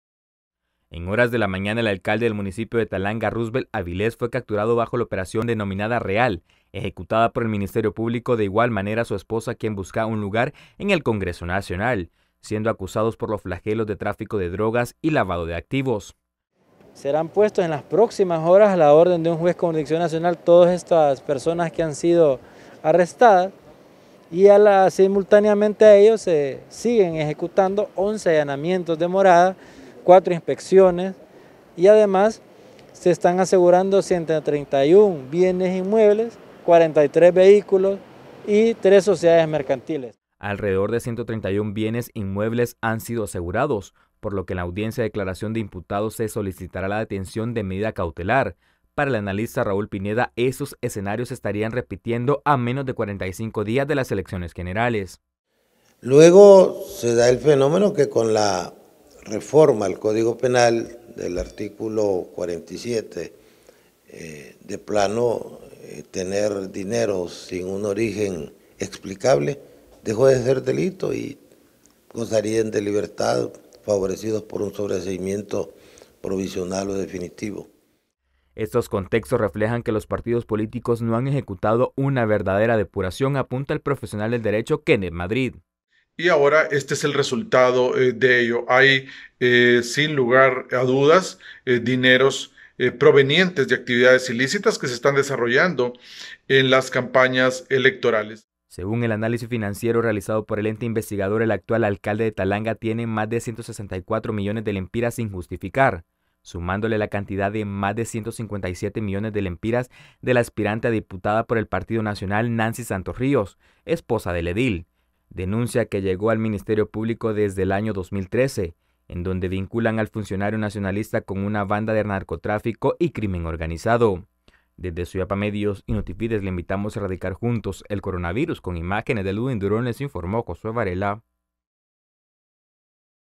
Captura del alcalde de Talanga
La captura del alcalde de Talanga junto a su esposa y demás implicados por flagelo de lavado de activos, es una lección para saber elegir a las próximas autoridades, aseguran analistas, quienes no descartan que se den más capturas en los próximos días.